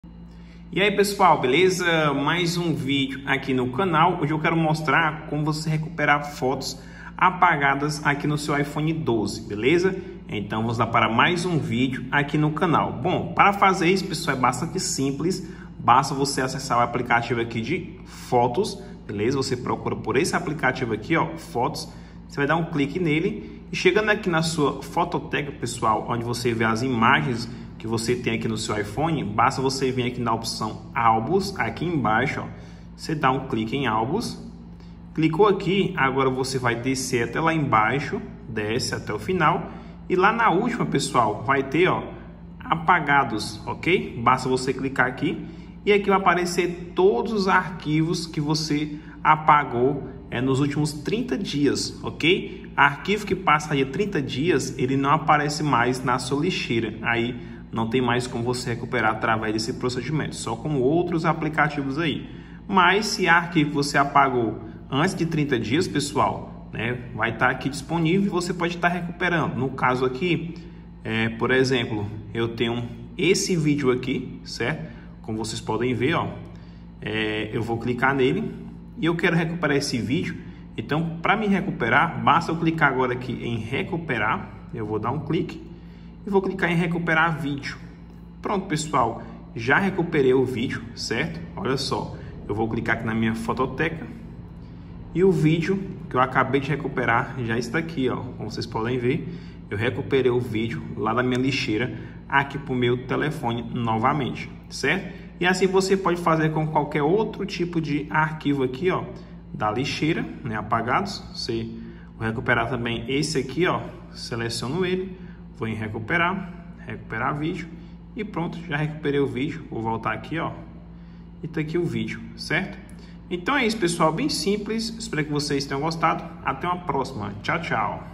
Como recuperar fotos e vídeos apagadas no iPhone 12
Fala pessoal!! vou mostrar passo a passo de como recuperar fotos e vídeos apagadas no iPhone 12

★ LINK ONDE COMPRAR:
★Moto G60

★ LINK ONDE COMPRAR:
★Moto G60